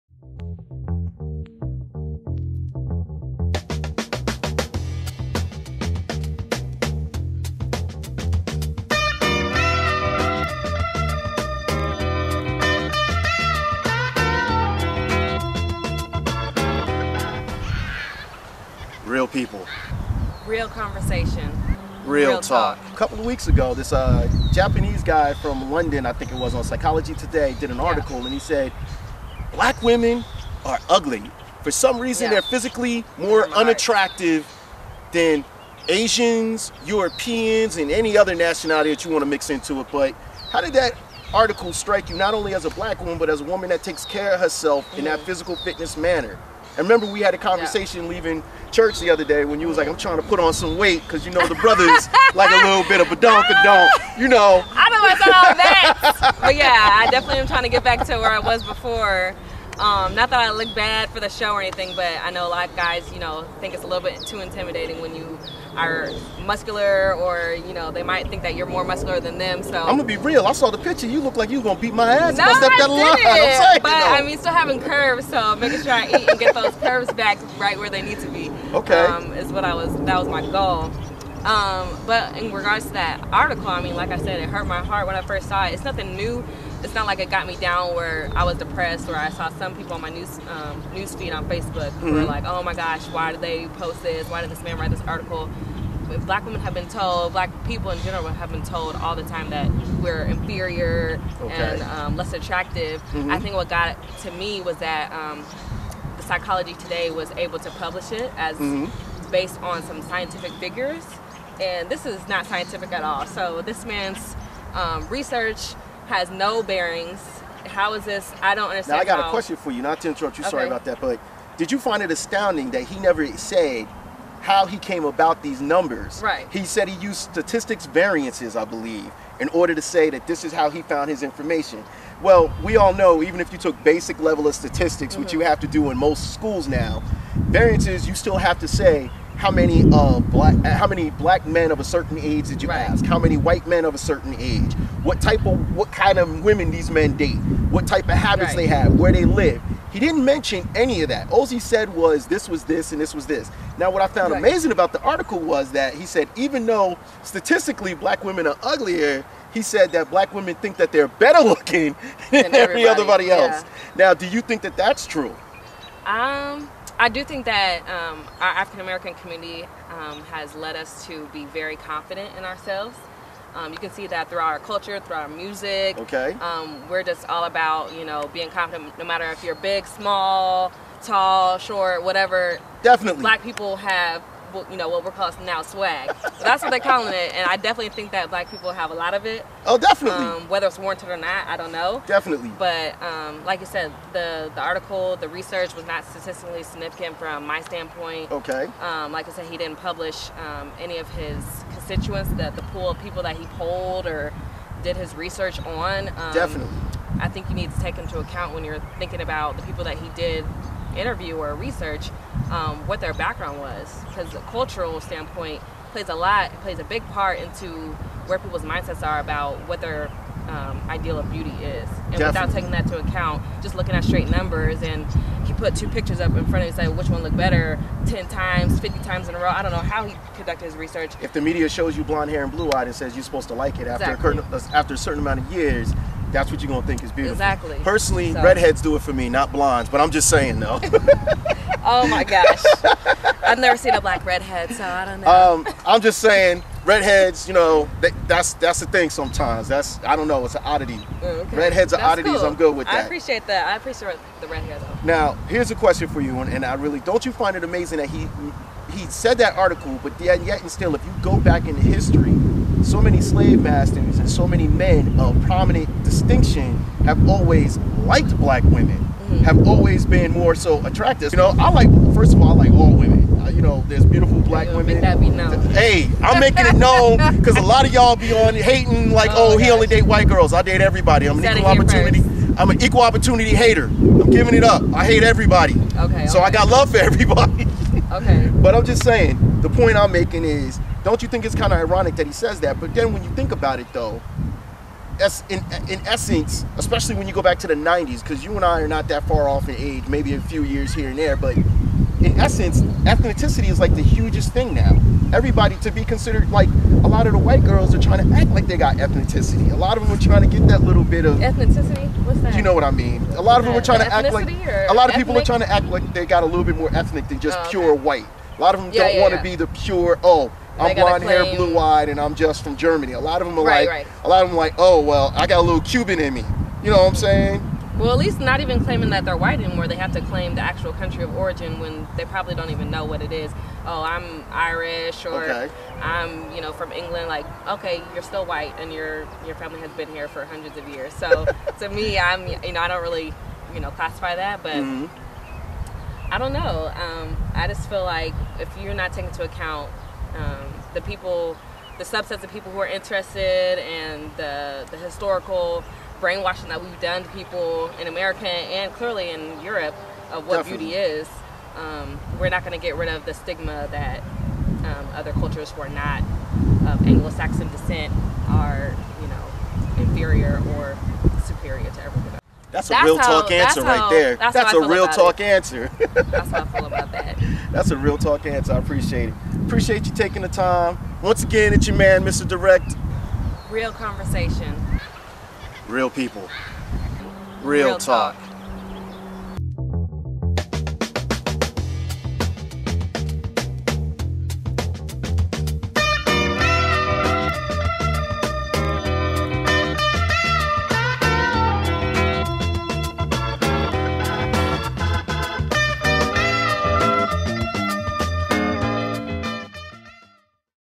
MisterDirect in Real Talk Episode 1
Are black women less attractive?  On May 7, 2011 Psychology Today published an article.  "Why are Black Women Less Physically Attractive Than Other Women?"  Dr. Satoshi Kanazawa offered scientific numbers to defend his argument.

The article gave scientific numbers to defend the opinion.  The article was the subject of much controversy.  Many more articles were written in rebuttal.